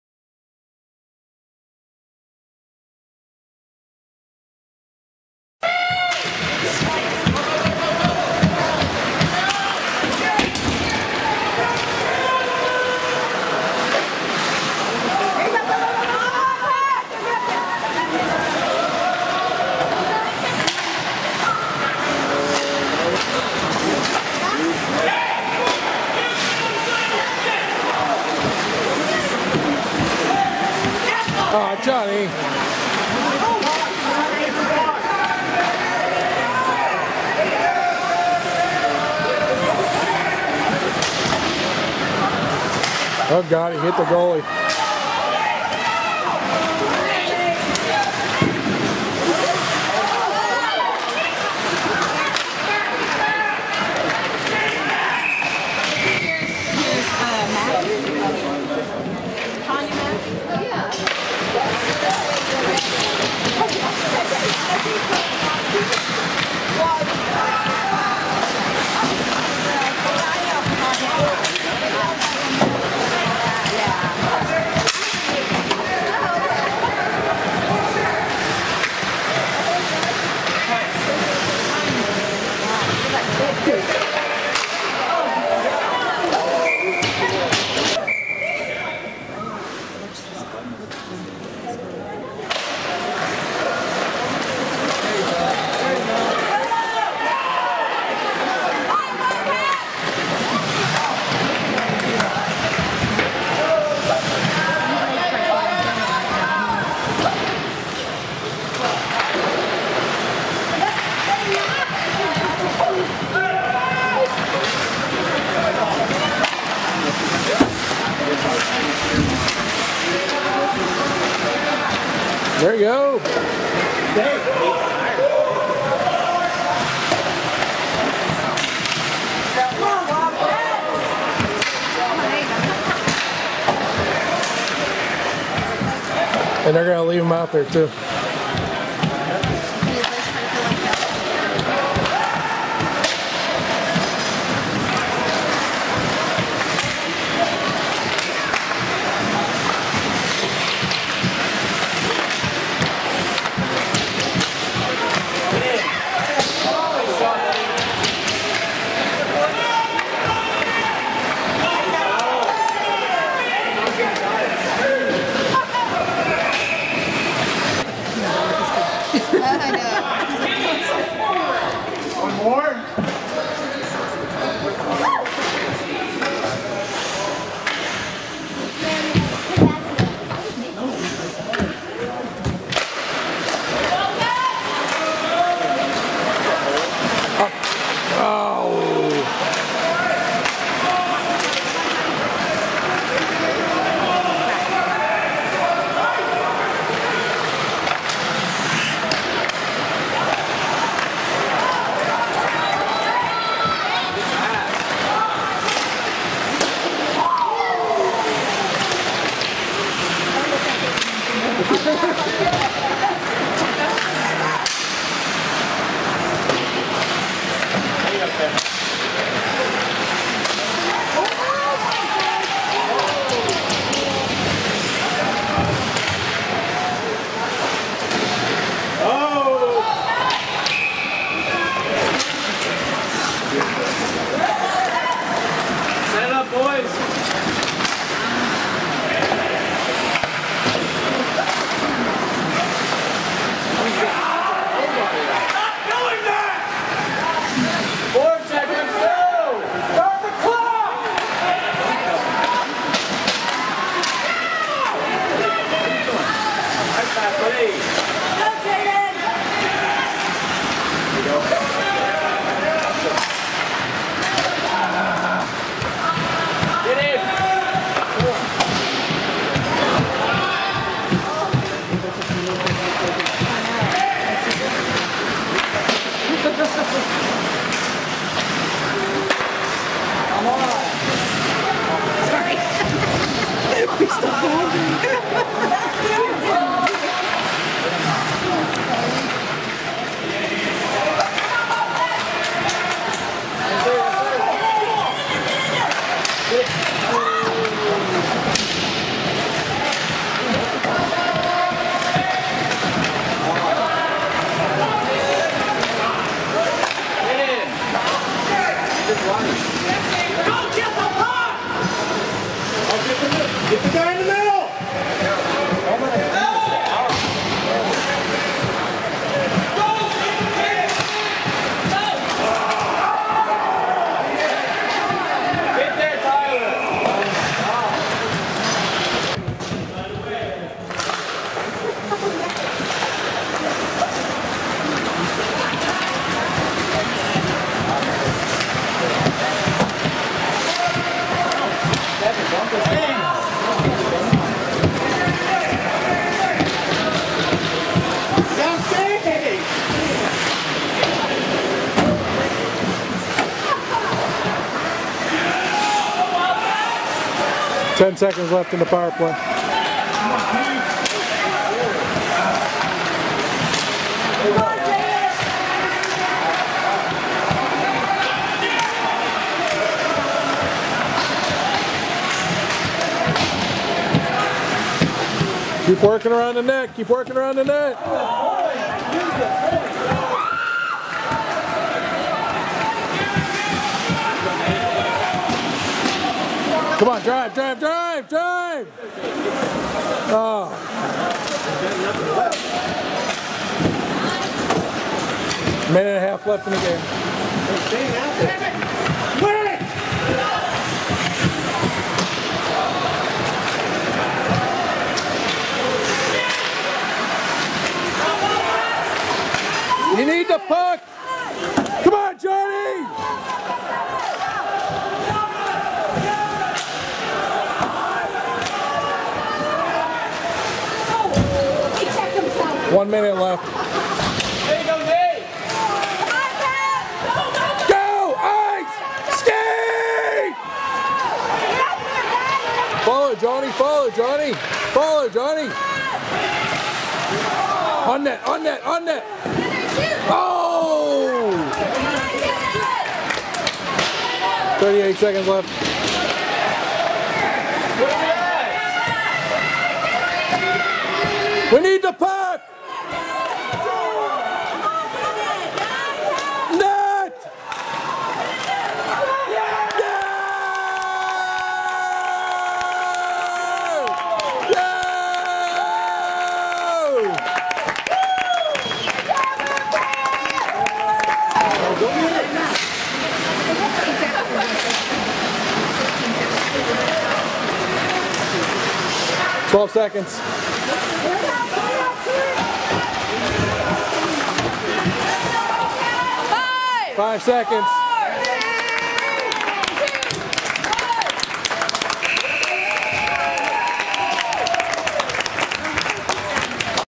Cy-Woods vs San Antone - ISHL Varsity Hockey - last 7:15 min - 03 01 2020
Cy-Woods - 5
San Antone - 5